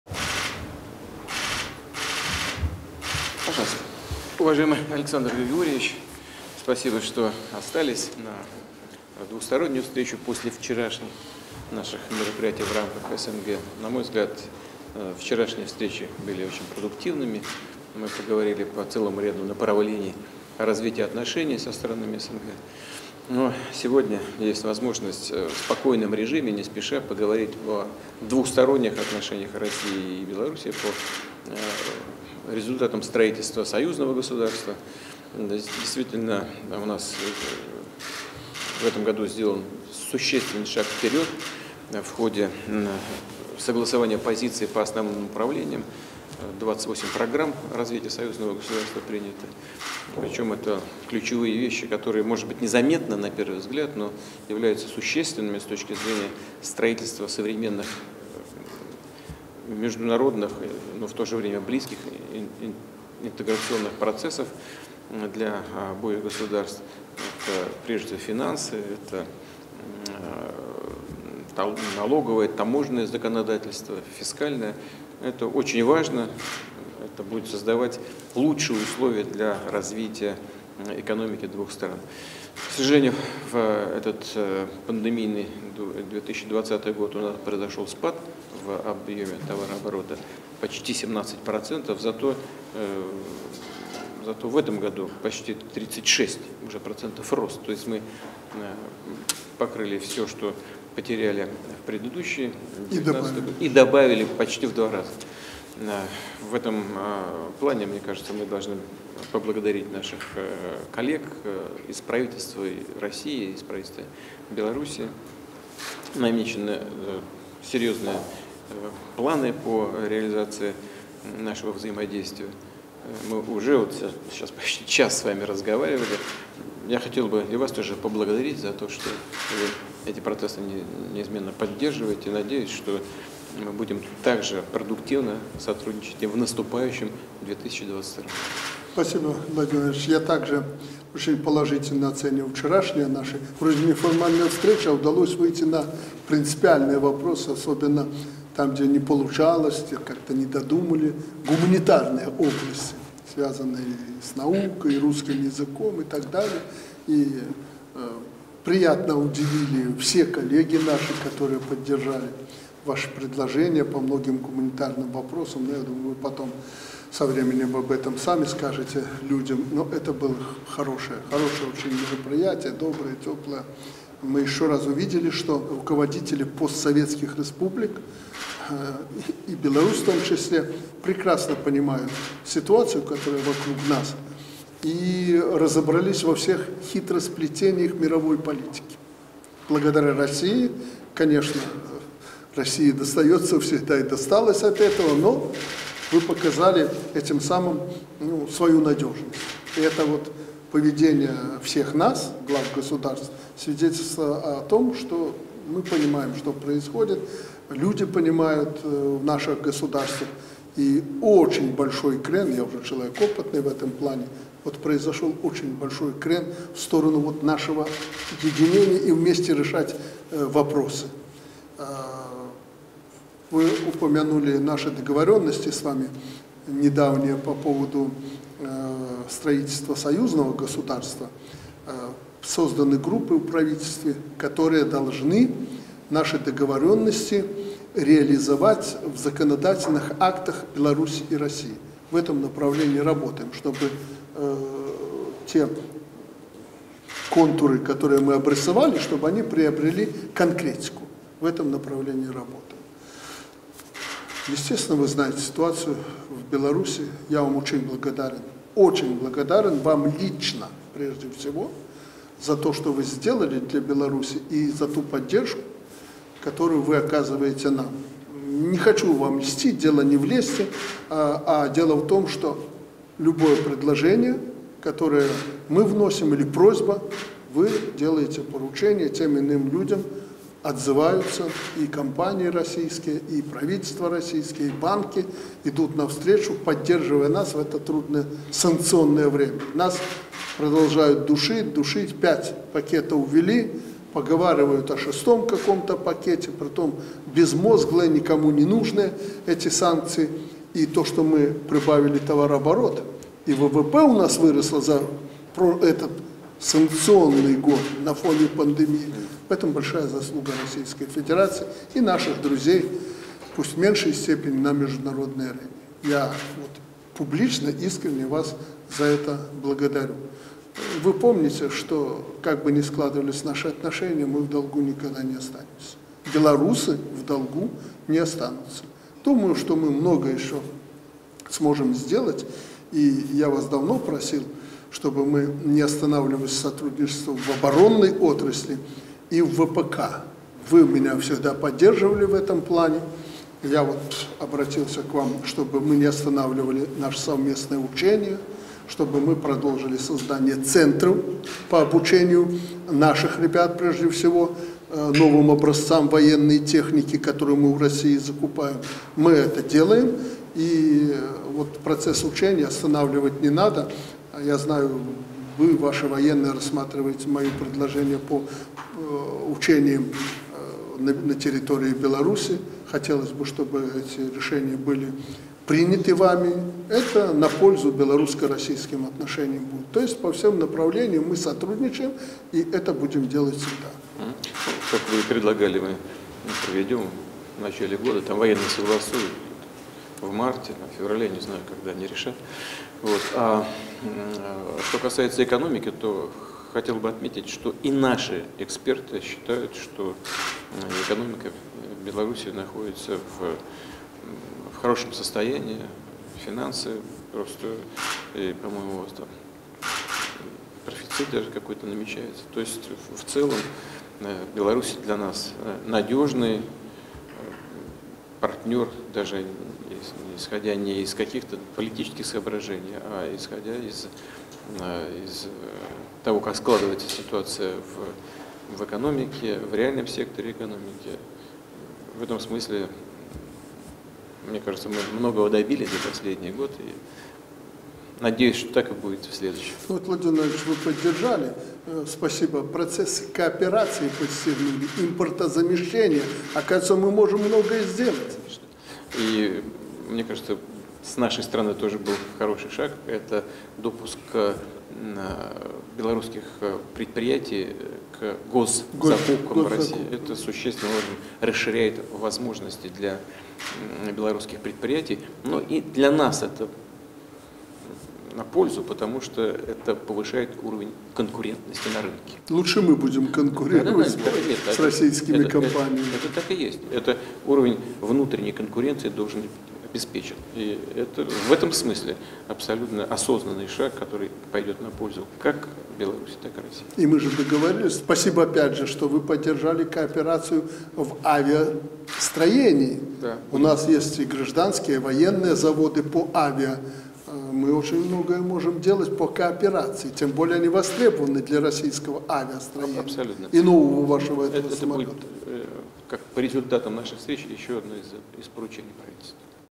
"Не хочу вам льстить! Дело не в лести!" | Встреча Лукашенко и Путина. Санкт-Петербург
Лукашенко - Путину: Естественно, вы знаете ситуацию в Беларуси. Я вам очень благодарен, вам лично прежде всего, за то, что вы сделали для Беларуси. И за ту поддержку, которую вы оказываете нам. Не хочу вам льстить. Дело не в лести. А дело в том, что любое предложение, которое мы вносим, или просьба - вы делаете поручение тем или иным людям, отзываются. И компании российские, и правительство, и банки идут навстречу, поддерживая нас в это трудное санкционное время. Нас продолжают душить. Пять пакетов ввели, поговаривают о шестом каком-то пакете. Притом безмозглые, никому не нужные эти санкции.

Президент Беларуси Александр Лукашенко поблагодарил российскую сторону, и прежде всего лично Президента Владимира Путина, за поддержку Беларуси в трудное санкционное время. Об этом он заявил на официальной части встречи с российским лидером в Санкт-Петербурге.